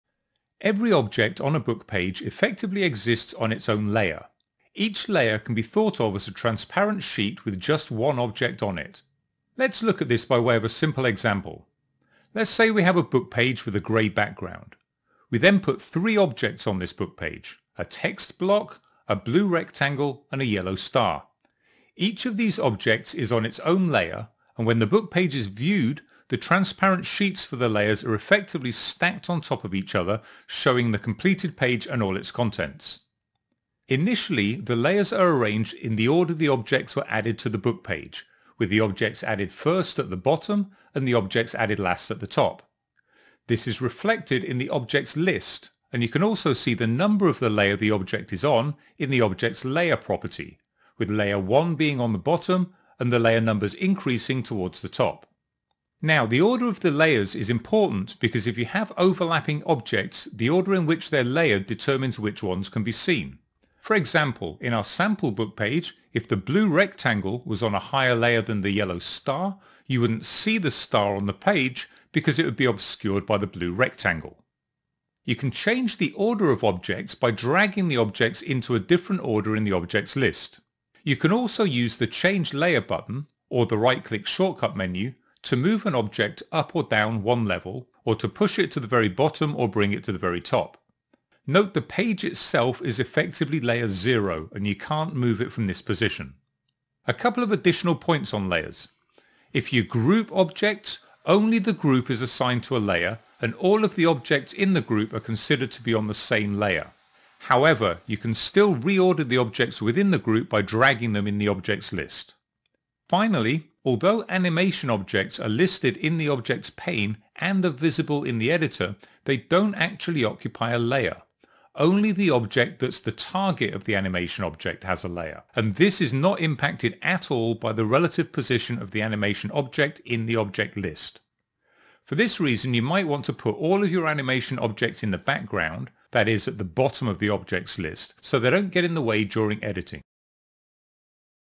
Understanding Book Page Layers
An extract from the course SEN-CW02 Adding Interest to your Book Pages, showing how layering works in SAP Enable Now. Techniques used on this page: Audio, Time Control, Visibility. (Simple video capture via SnagIt)